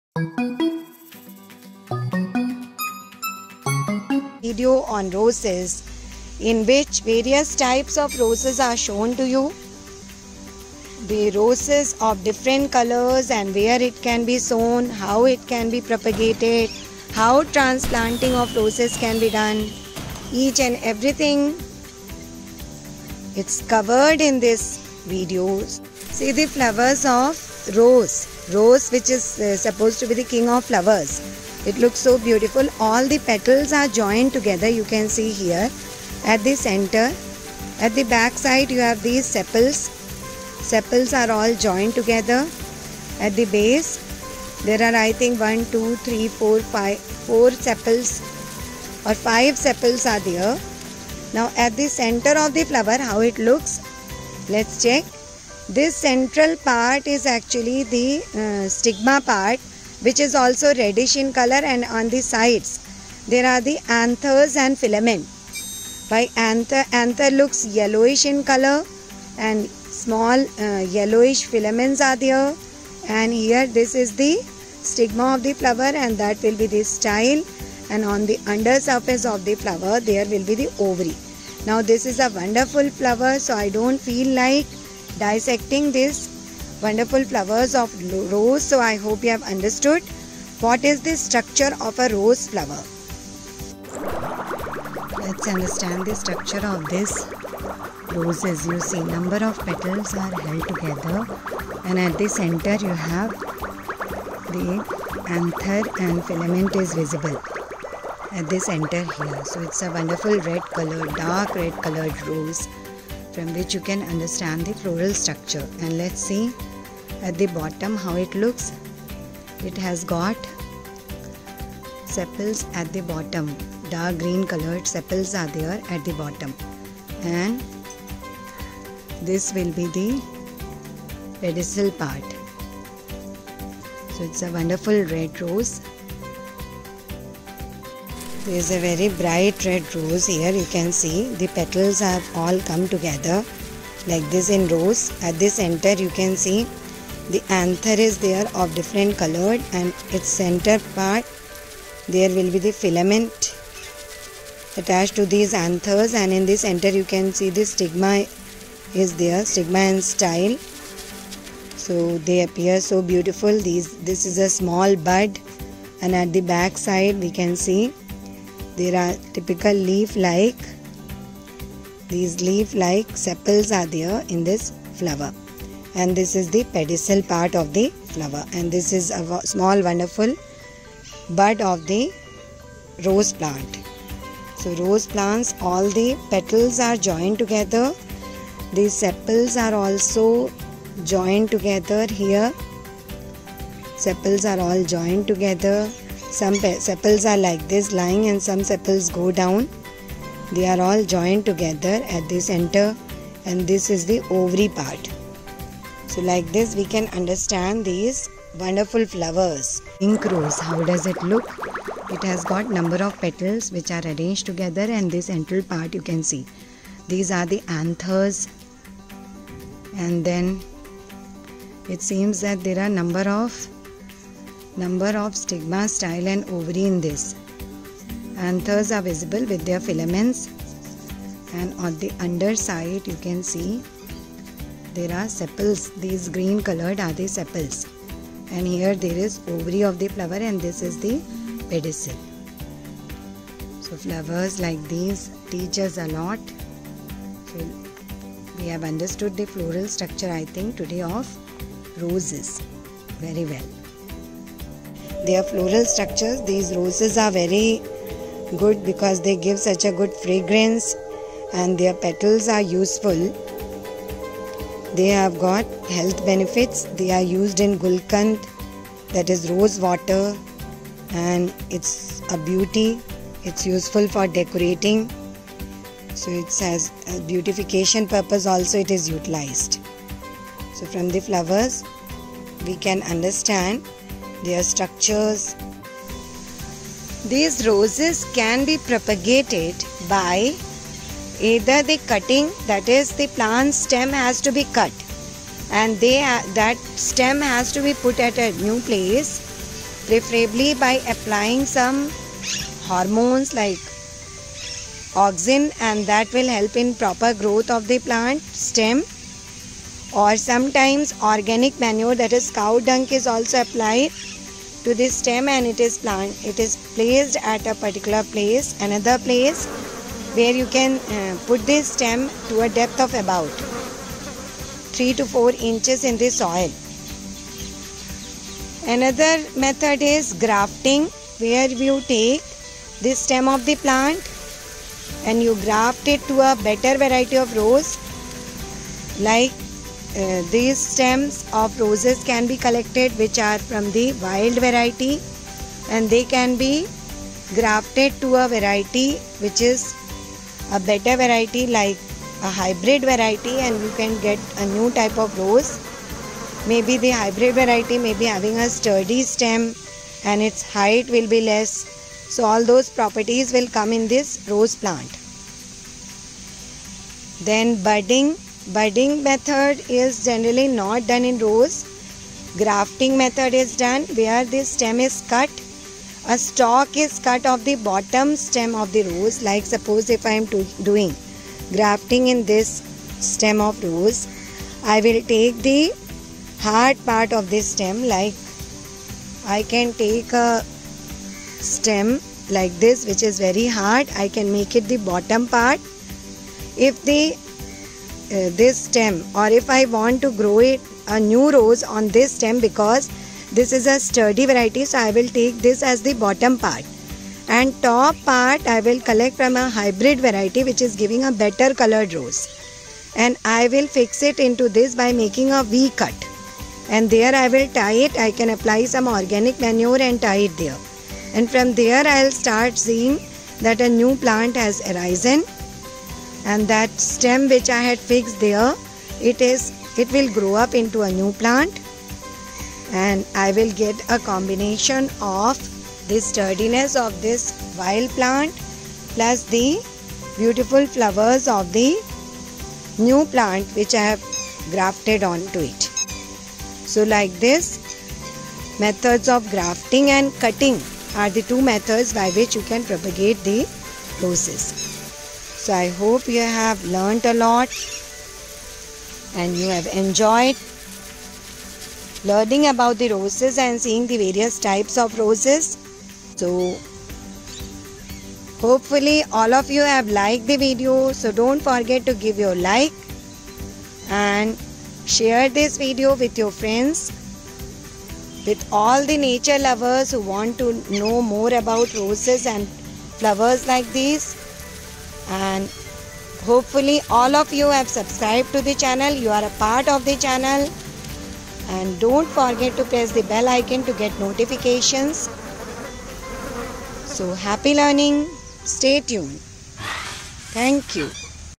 Beautiful Roses|Floral Structure|How to propagate| Colors|Uses|Gardeners|Homemakers|Student|3D Video
All gardeners, students, and nature lovers will love this video explaining parts of flowers of roses, different colored roses, and how this plant can be propagated.3 D effects are added to this video, music is freely provided and the video is informational to know about rose parts, uses, and propagation. Homemakers can try to follow the methods of propagation to grow more colorful rose plants.
Follow the Unique learning Channel to get such videos on nature, Science, Maths, and English. Give your wonderful comments and likes.